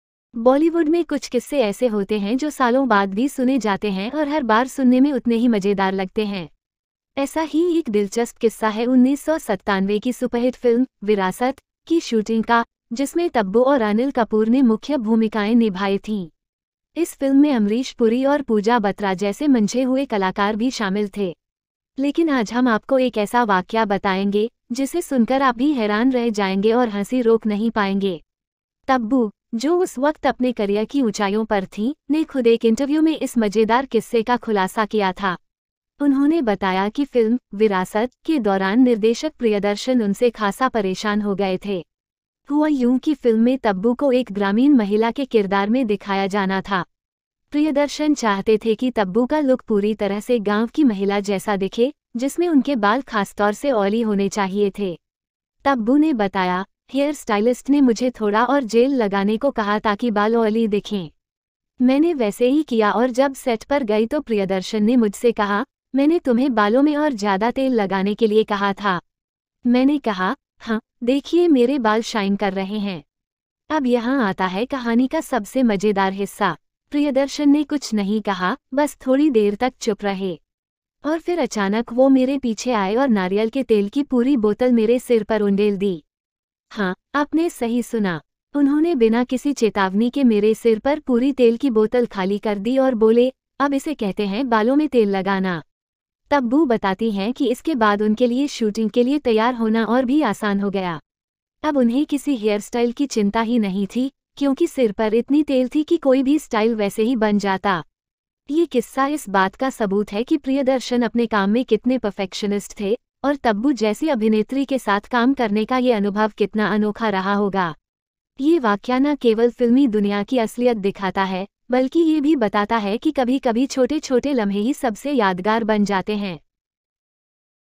जब तब्बू के सिर पर प्रियदर्शन ने पलट दी थी तेल की पूरी बोतल, जानिए वो मजेदार किस्सा
bollywood old love stories movies, bollywood old story movies, bollywood old stories